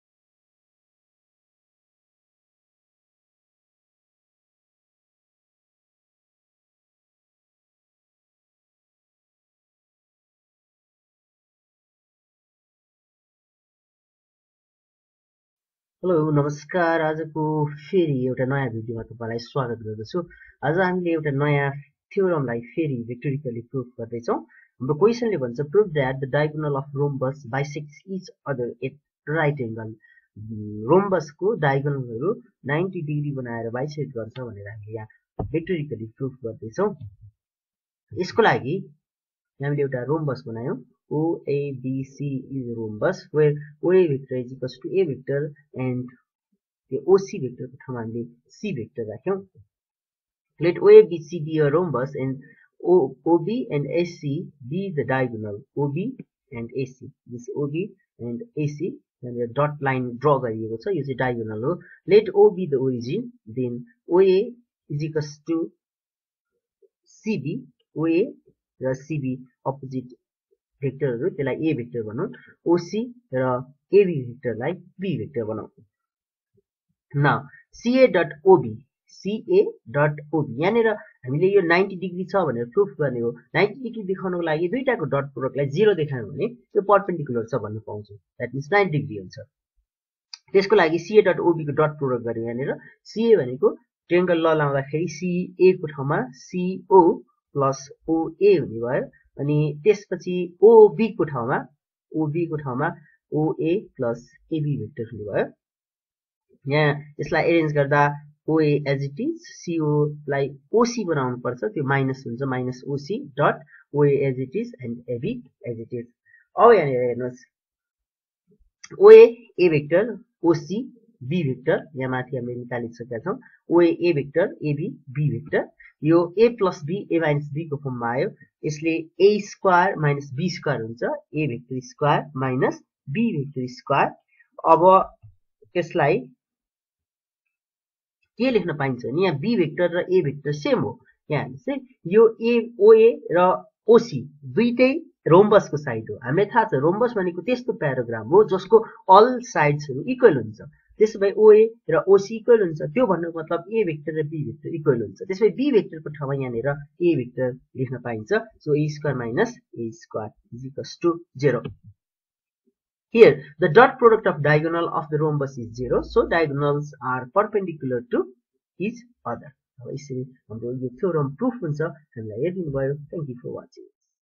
Vector Theorem 12: Prove that the diagonal of rhombus bisects each other at right angle.
Prove that the middle point of hypotenuse of a right angled triangle is equidistant from its vertices